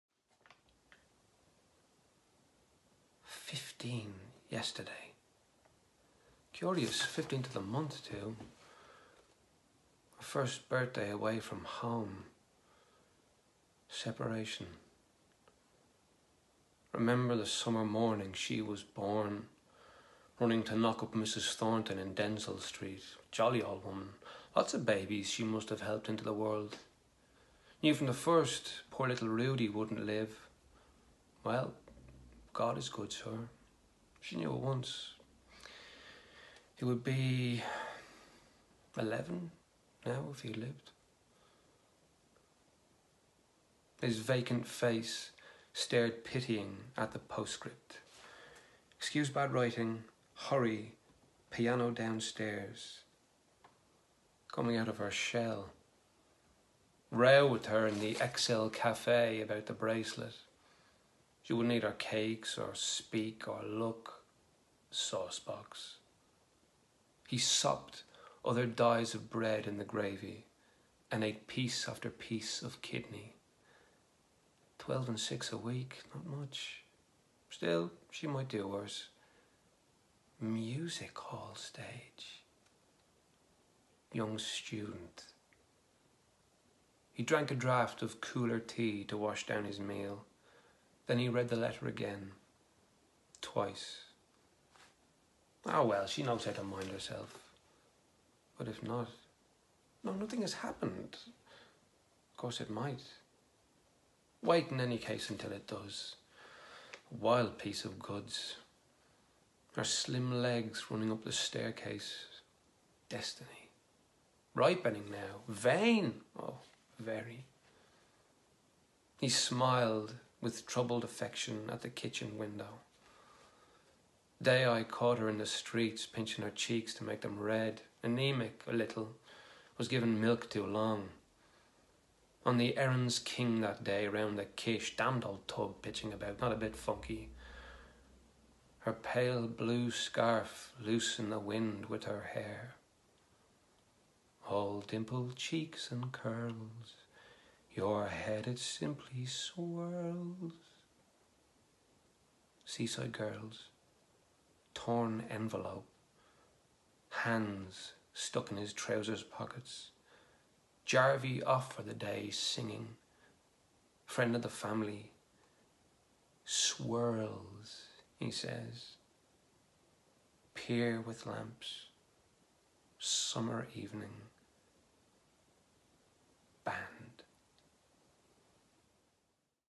Tom Vaughan-Lawlor – Episode 4/Calypso; Leopold ruminates on Millie’s letter.
The next in Embassy London’s Bloomsday 2020 celebration.